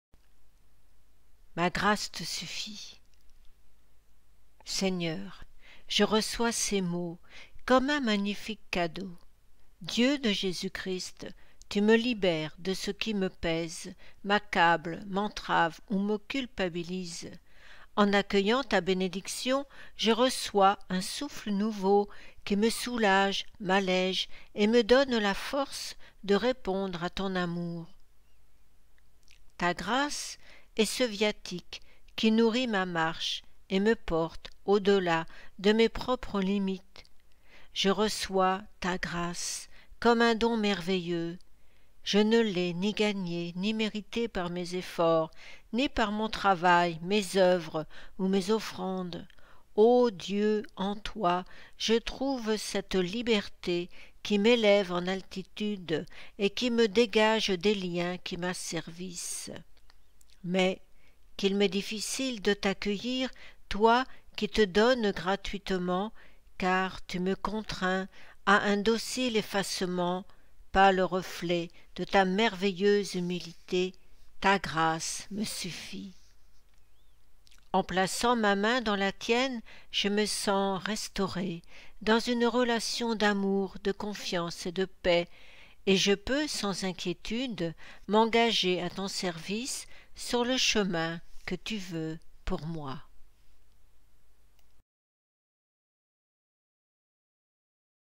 Ma grâce te suffit .
2 Corinthiens 12.9
Envoi
En accueillant ta bénédiction
je reçois un souffle nouveau.
Poésie Claude Caux-Berthoud
Voix Anne Miller